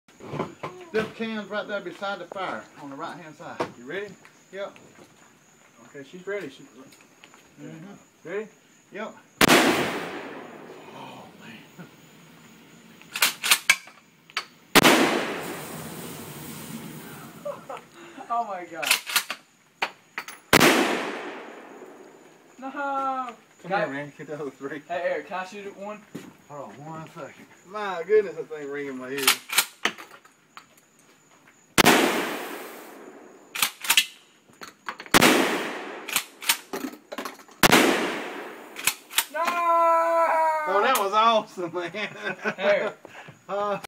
Steve Gregory shooting 12 gauge
Steve shooting Paint cans and soda bottles with 12 gauge shootgun